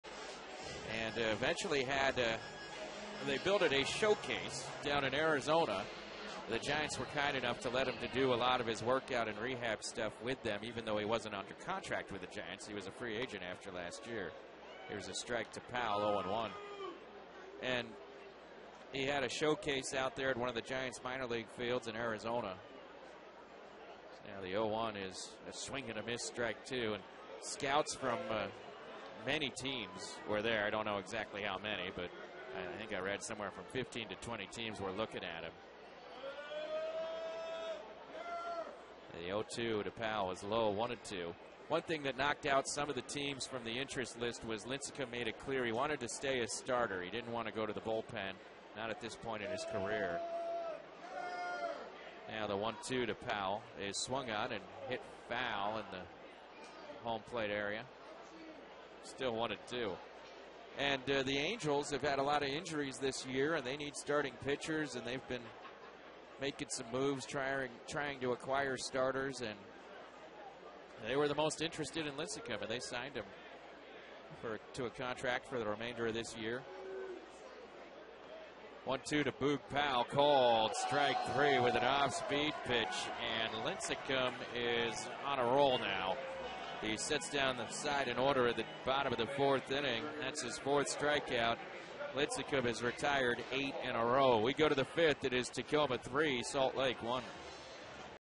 Salt Lake's Lincecum strikes out Tacoma's Powell
6/2/16: Salt Lake's Tim Lincecum rings up Tacoma's Boog Powell in the fourth inning of his first start in the Angels system